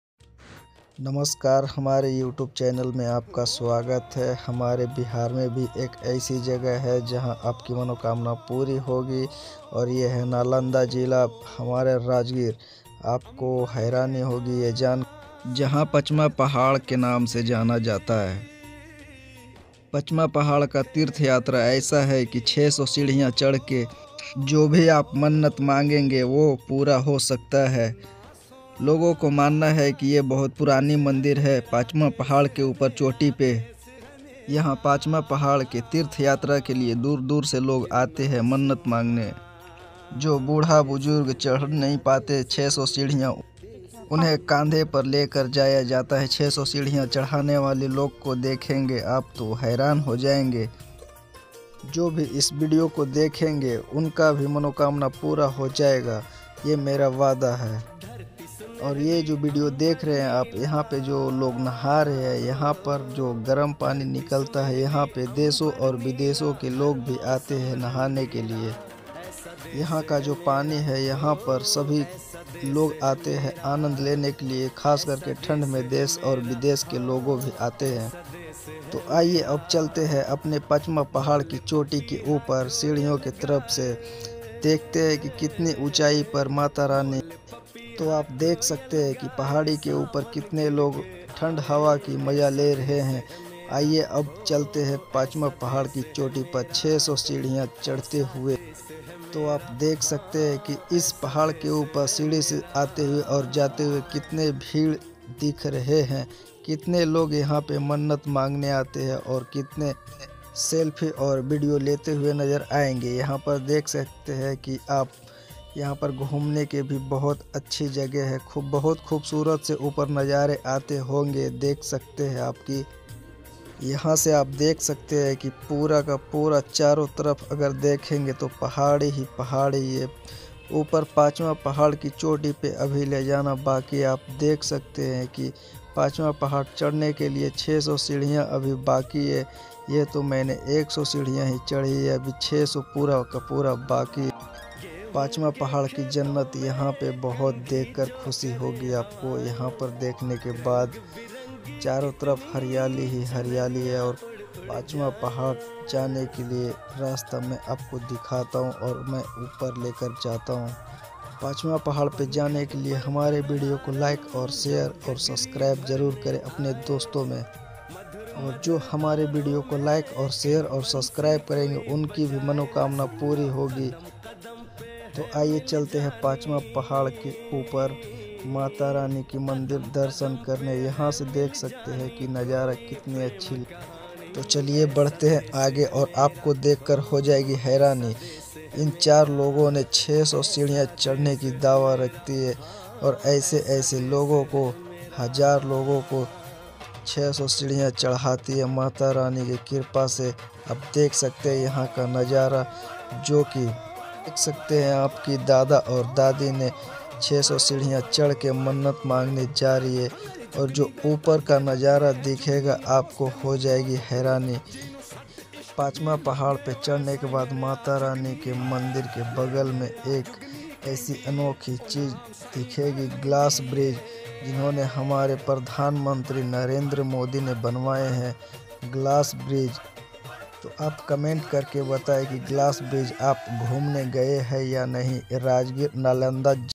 नालंदा राजगिर में उपस्थित है 600 सीढ़ियां की यात्रा कंधो पर पहाड़ियों के बीच आपको देखकर होगी हैरानी
नालंदा  राजगिर में उपस्थित है 600 सीढ़ियां की यात्रा कंधो पर पहाड़ियों के बीच आपको देखकर होगी हैरानी narhat jitendar bhai